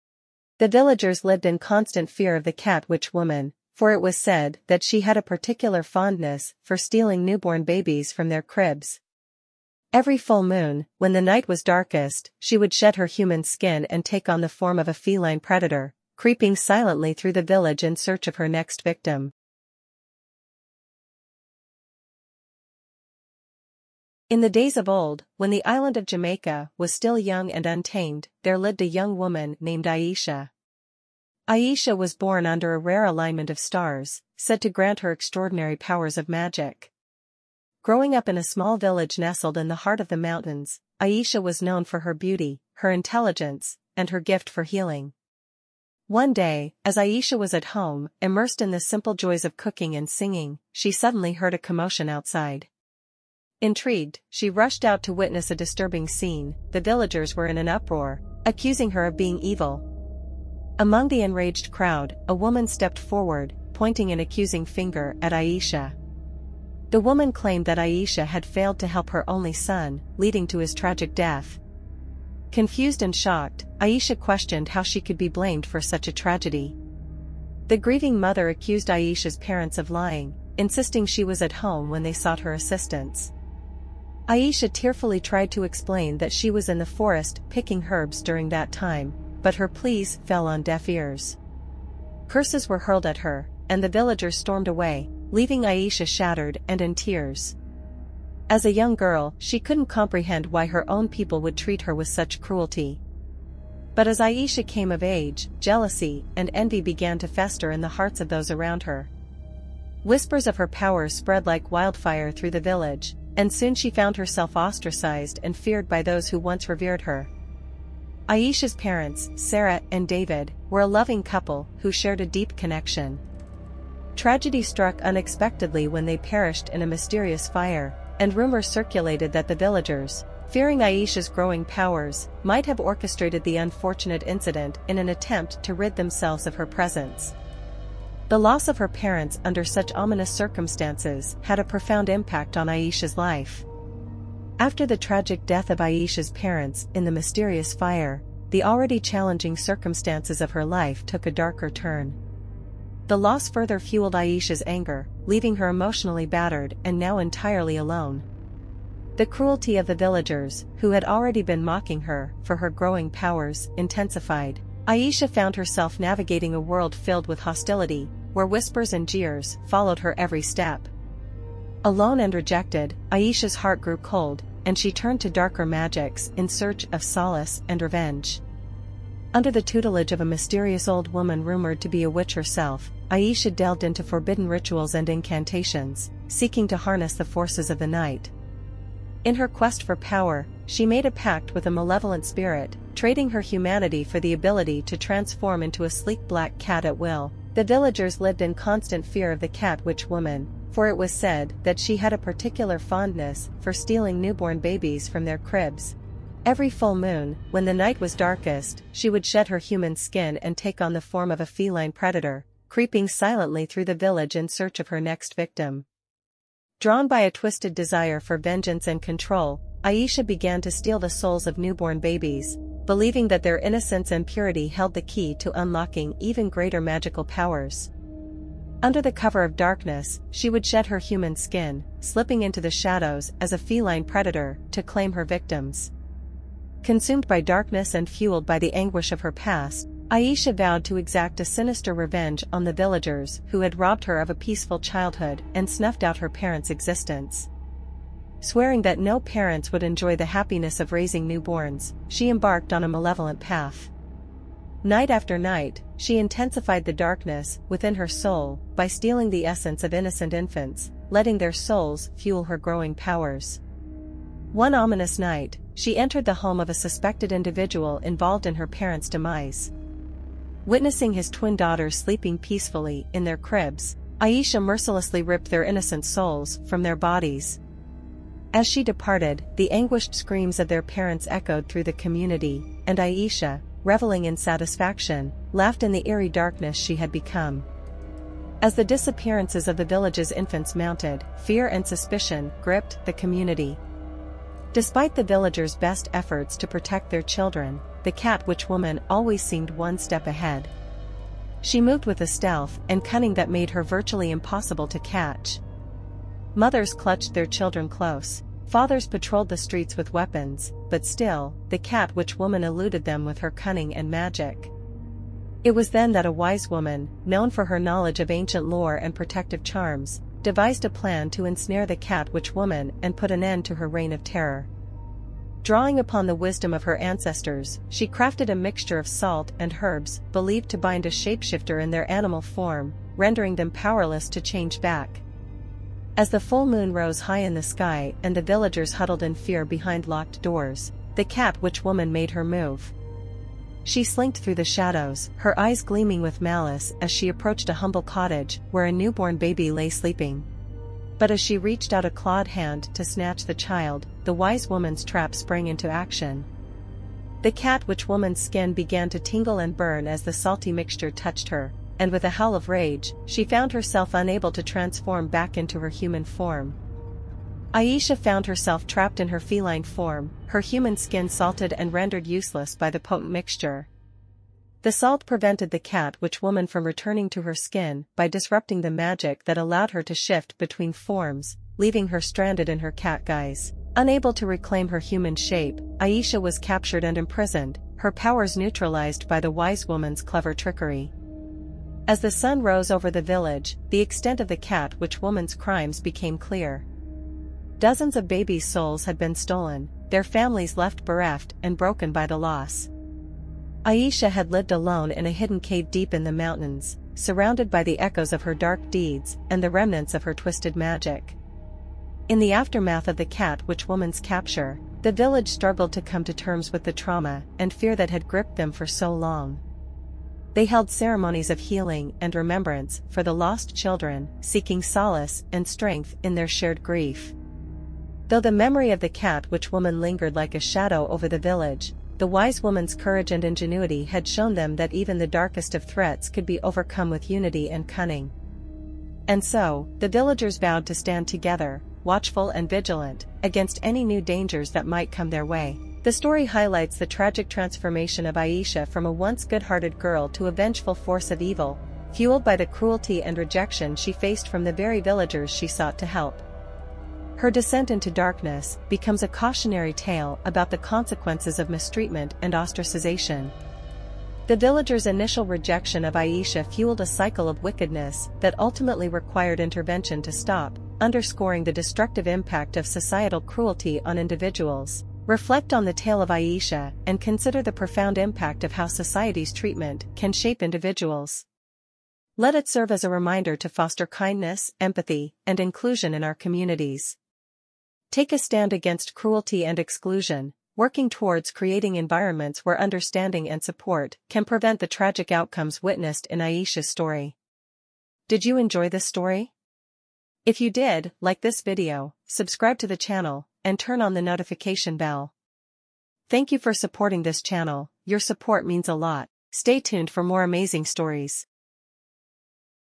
The Shape shifting Cat Witch and the Stolen Souls (Ol' Hige)
Description: Embark on a chilling journey into the heart of Jamaican folklore with "The Shape-shifting Cat Witch and the Stolen Souls."" This captivating script unfolds the tragic tale of Aisha, a once-innocent girl, scorned and driven to dark magic by the cruelty of her own village. As she transforms into a malevolent cat witch, her thirst for vengeance leads to the haunting theft of newborn souls. Set against the backdrop of Jamaica's mystic landscapes, this story weaves together elements of shape-shifting, folklore, and the resilience of an orphaned soul. Brace yourself for a spellbinding narrative that explores the consequences of rejection, the allure of forbidden magic, and the enduring power of unity in the face of supernatural malevolence. JamaicanFolklore Ol' Hige